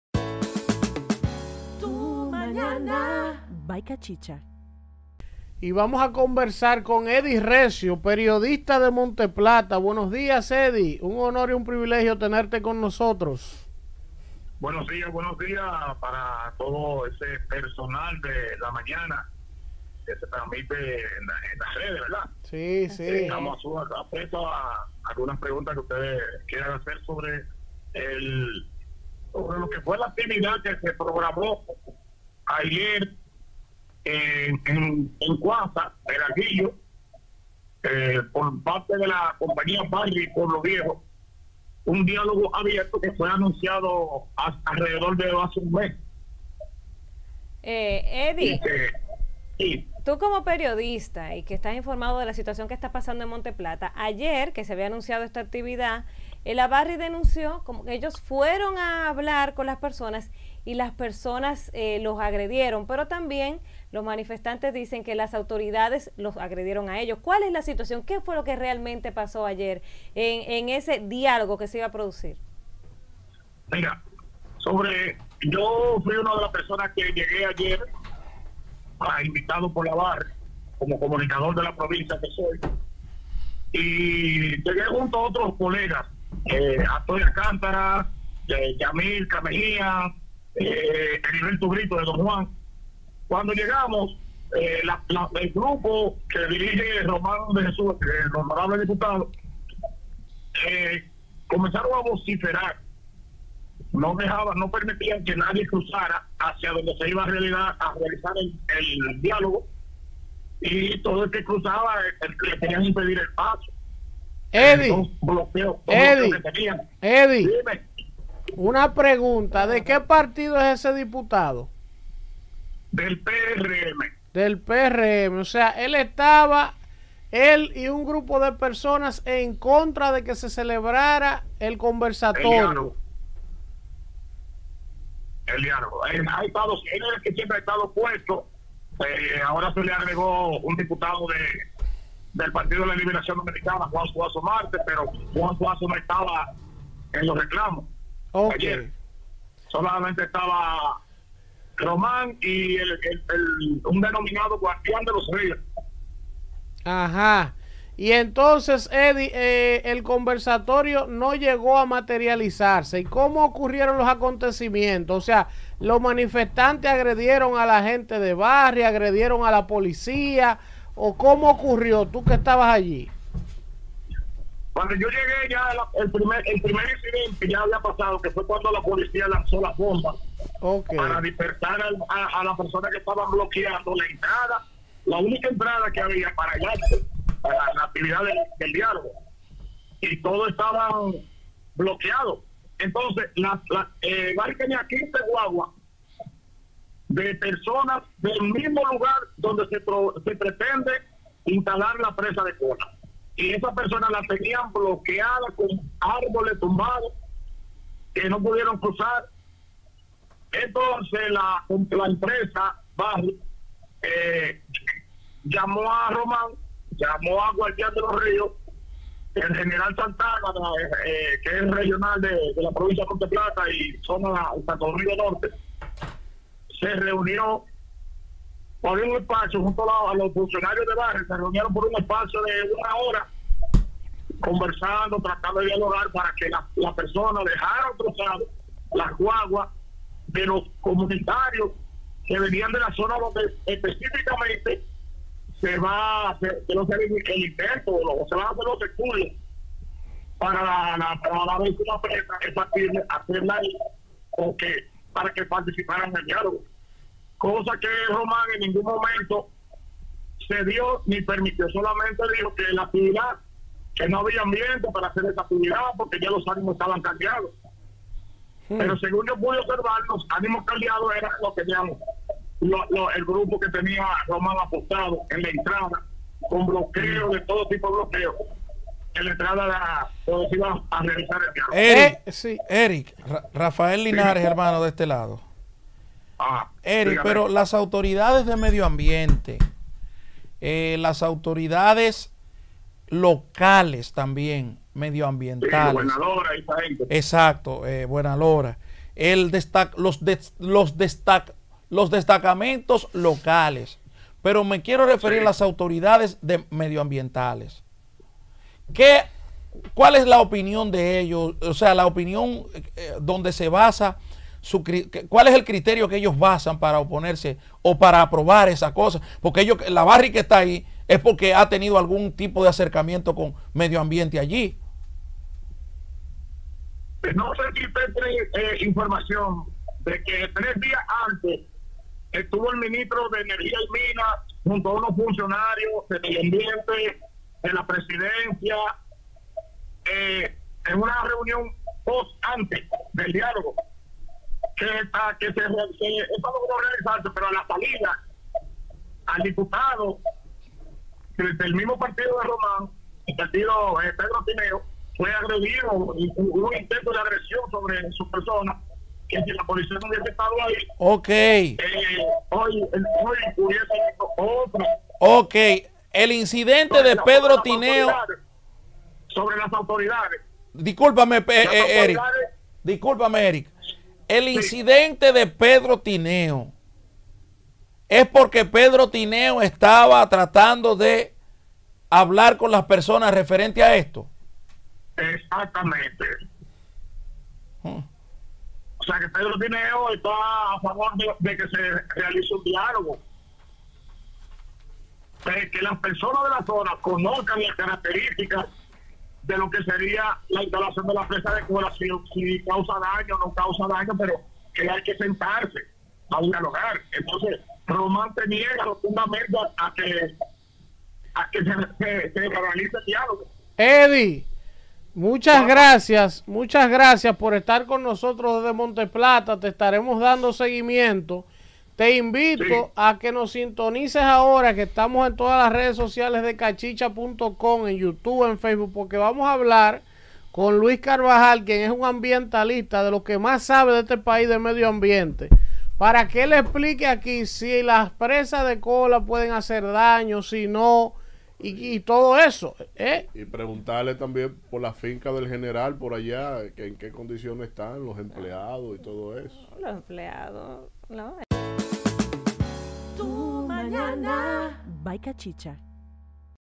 Incidentes en Barrick Pueblo Viejo
Barrick Pueblo Viejo deploró los incidentes registrados este jueves en La Cuaba, Peralvillo, Monte Plata, donde un grupo de personas abortó el intento de un diálogo con la comunidad para buscar avenencia, llevar orientación y entendimiento en los planes de expansión de la empresa minera hasta esa demarcación.

En su versión de los hechos, Pueblo Viejo informó que un grupo de personas violentaron el derecho constitucional de otras interesadas en participar de forma pacífica en un diálogo convocado por la minera para concertar y socializar con la población de Yamasá y de otras comunidades aledañas.